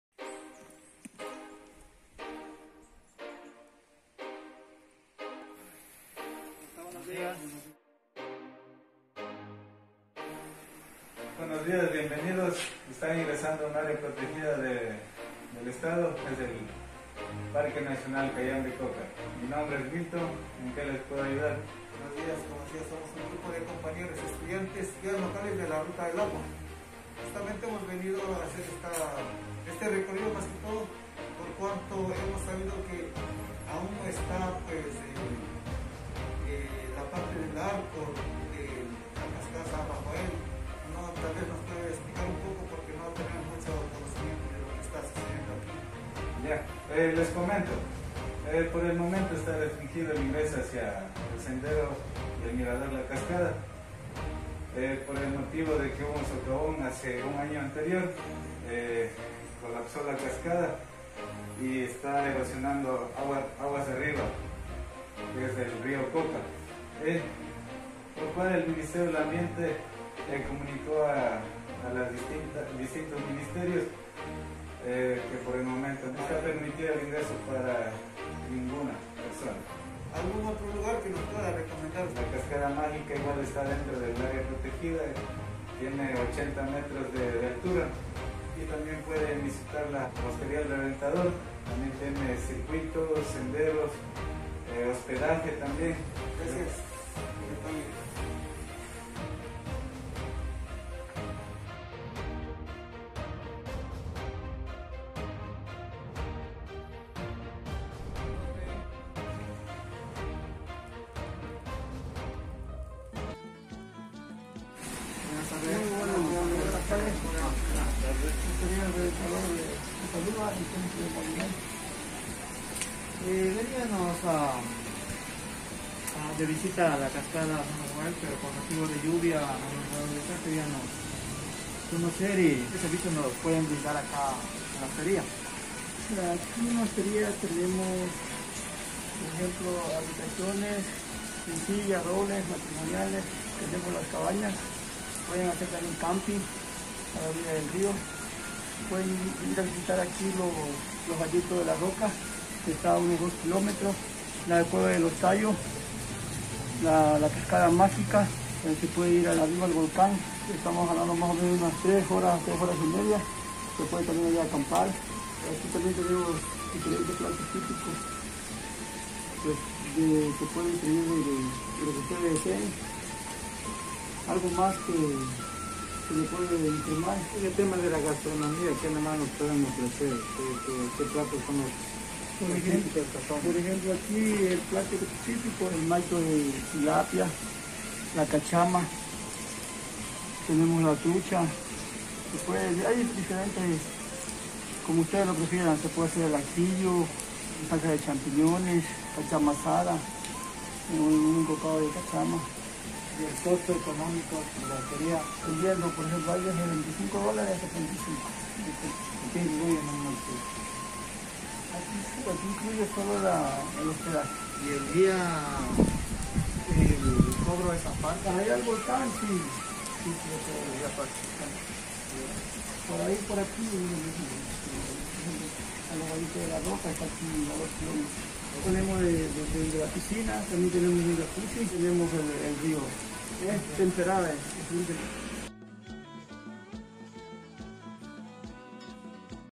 LIDERAZGO Y MANEJO DE CONFLICTOS MODULO 6
Este material está dedicado con fines educativos, cuya área de investigación los hicimos en los predios del Parque Nacional Cayambe Coca, sector San Rafael, como también en la Hostería el Reventador, entrada a la provincia de sucumbíos vía a Quito, km 159, enfocados en el liderazgo y atención al cliente, ya que a esos sitios acuden con frecuencia los turistas nacionales y extranjeros, trabajo elaborado grupalmente por : Bernardo Jimbo, Cleber Andi, Humberto Castillo, Roger Farinango, Xavier Salazar, estudiantes para GUIAS LOCALES DE LA RUTA DEL AGUA, del Cantón Gonzalo Pizarro. Un agradecimiento profundo a la Magister: Diana Enríquez, Docente de la UNIVERSIDAD CENTRAL DEL ECUADOR y a su equipo de colaboradores quienes nos enriquecieron con sus conocimientos. Edición y producción: Cleber Andi Vargas 2021.